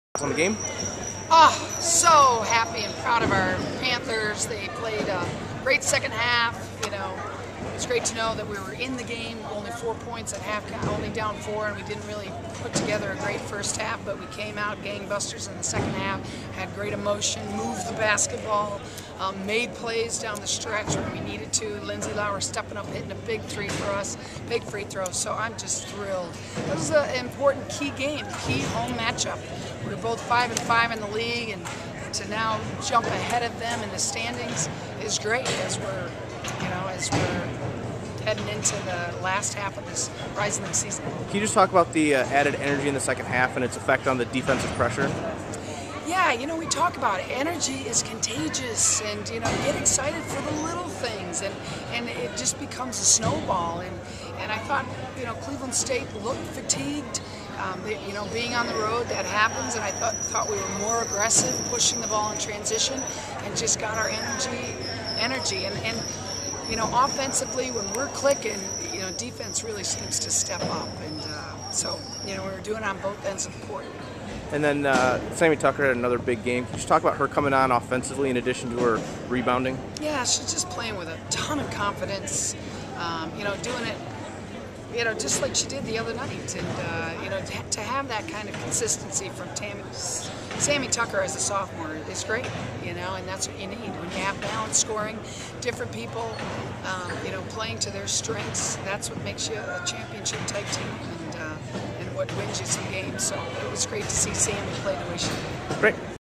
WBB Post-Game: Cleveland State
Milwaukee head coach Sandy Botham talks about the Panthers' 73-64 win over Cleveland State Feb. 5, 2011.